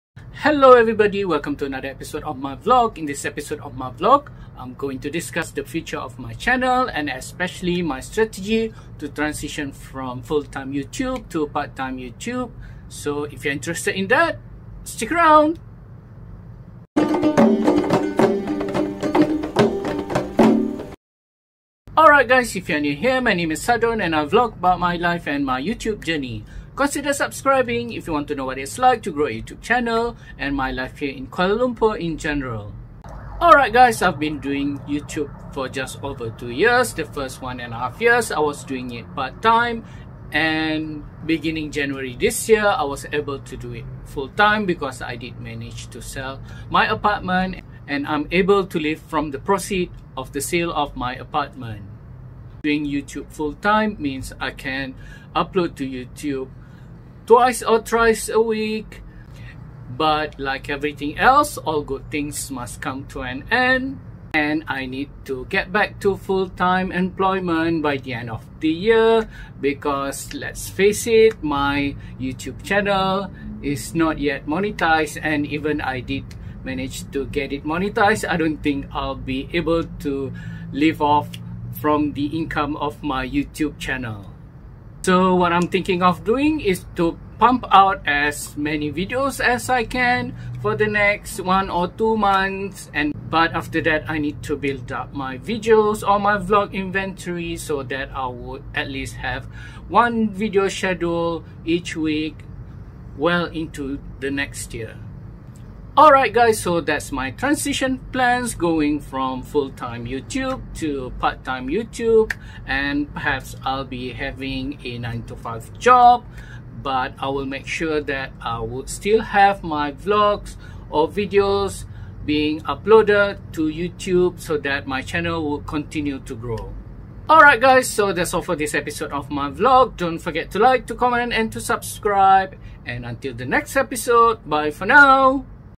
How to manage a channel with a fulltime job - my strategy for parttime YouTube | Vlog 175
How to manage a channel with a fulltime job - my strategy for part-time YouTube | Unscheduled vlog 25

In this episode the vlogger shares his strategy to transition to part time YouTube after doing it full time for six months. He had to go back to full time employment within the next six month after failing to blow up on YouTube doing it full time for the last six months.